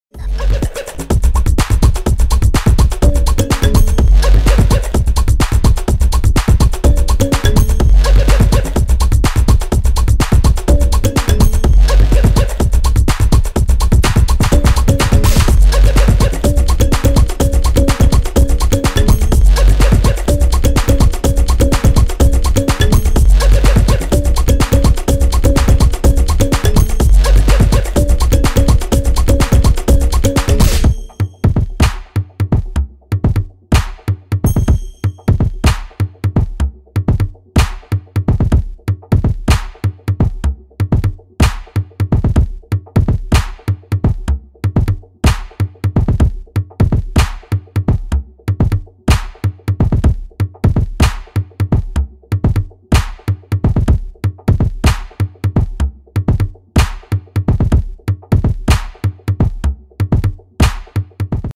Vinny Arora super hot Yoga pose in pink joggers makes hubby Dheeraj Dhoopar go lovestruck in public
Check out Vinny Arora's latest photo